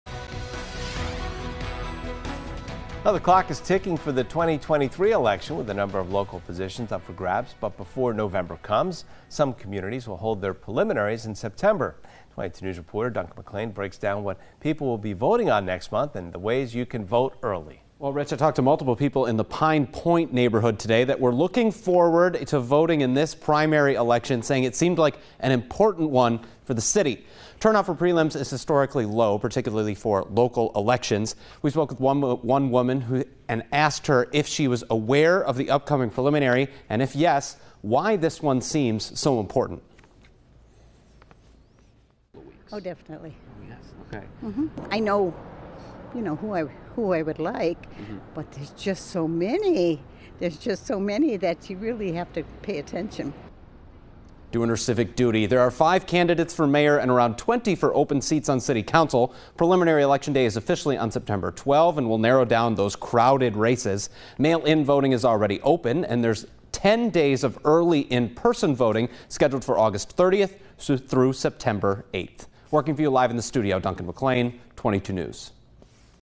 Early voting for preliminary election in Springfield open
SPRINGFIELD, Mass. (WWLP) - A pivotal preliminary election is just over two weeks away in Springfield and the city is ready.